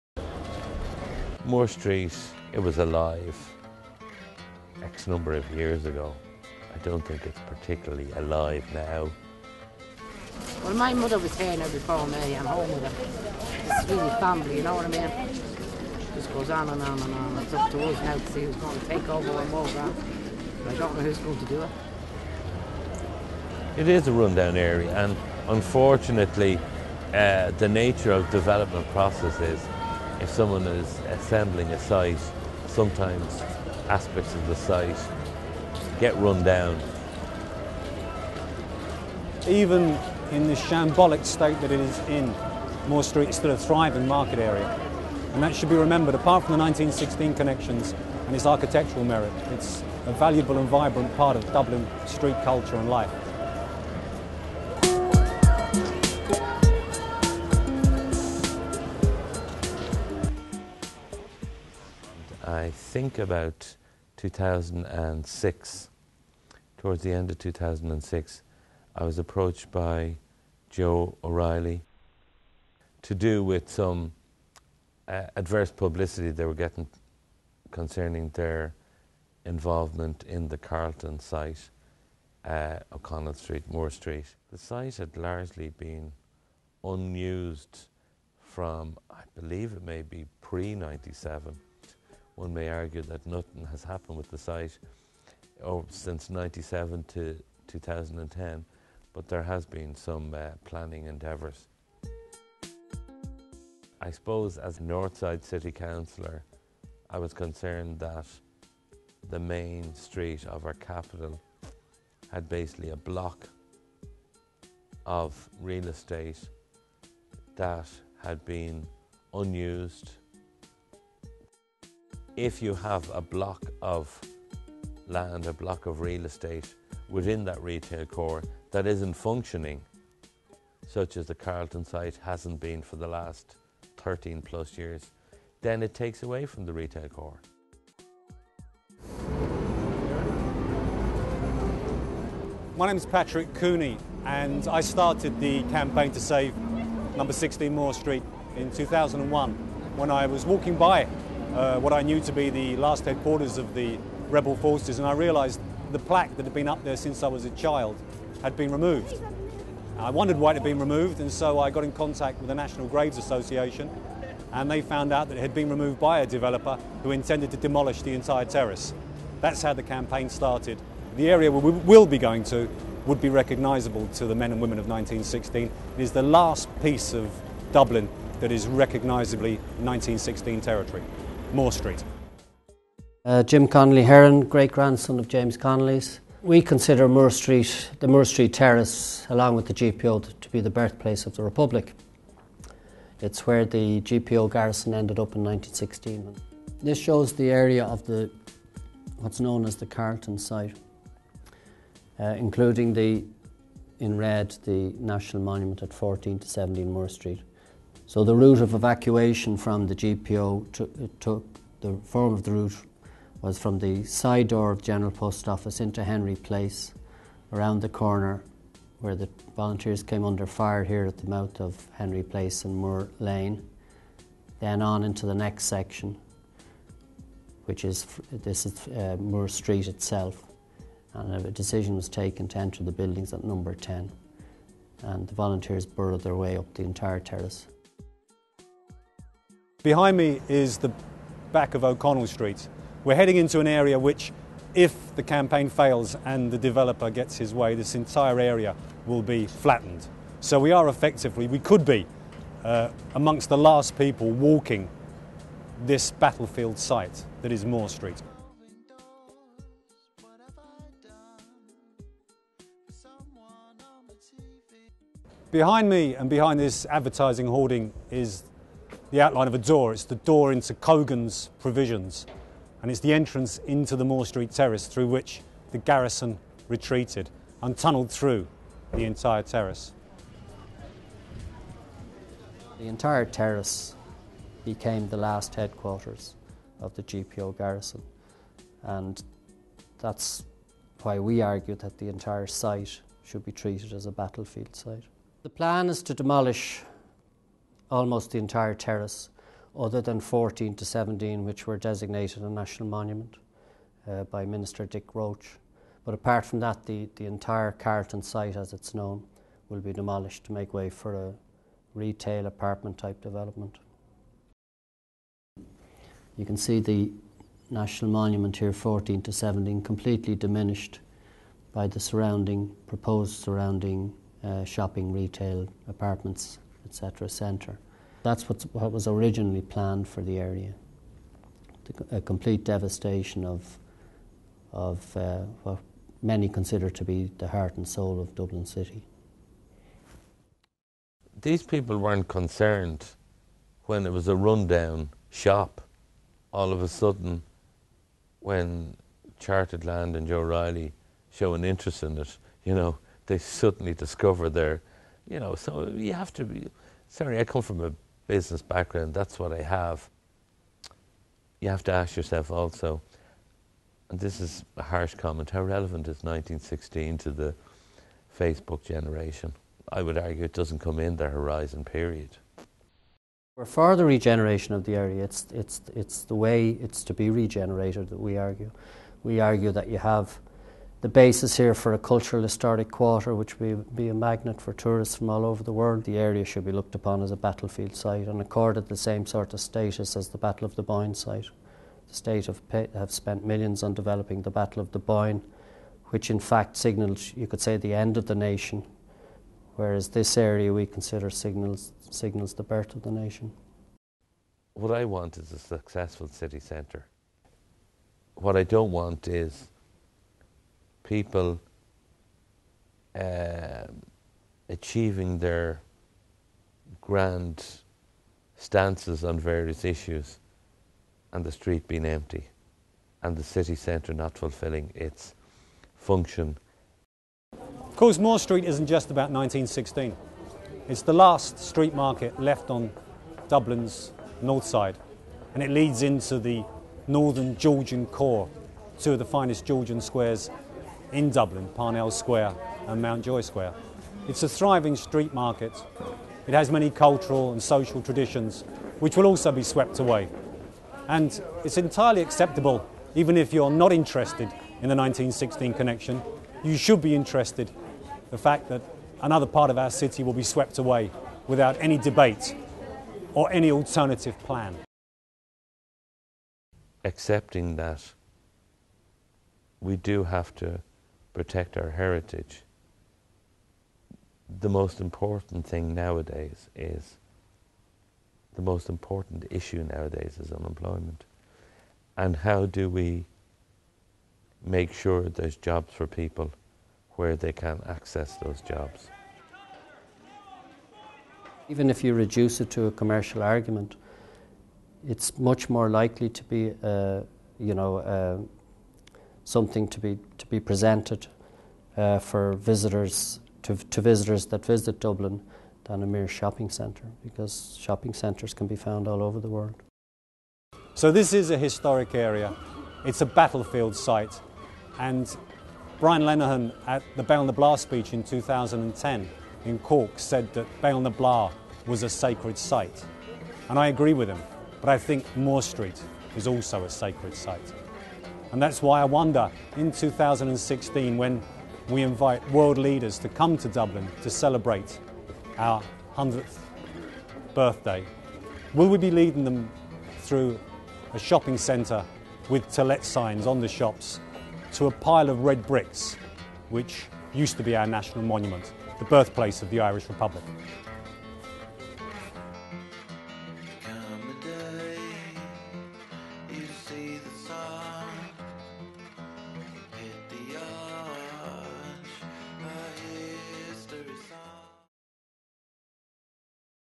Moore street - An uncertain future
A short documentary on the development of Moore street in Dublin city. We speak with Patrick Cooney,  James Connolly Heron and the Lord Mayor of Dublin.